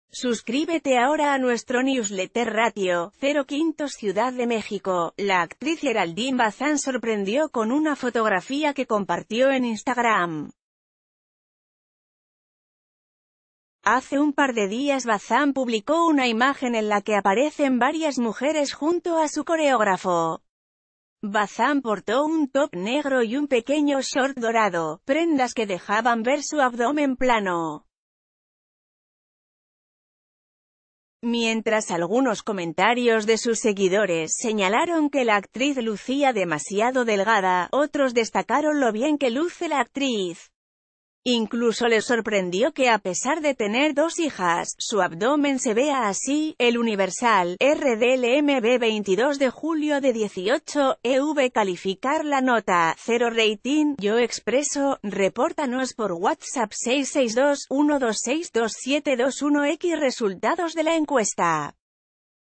Geraldine Bazán presume su esbelta figura en redes
Hace un par de días Bazán publicó una imagen en la que aparecen varias mujeres junto a su coreógrafo. Bazán portó un top negro y un pequeño short ...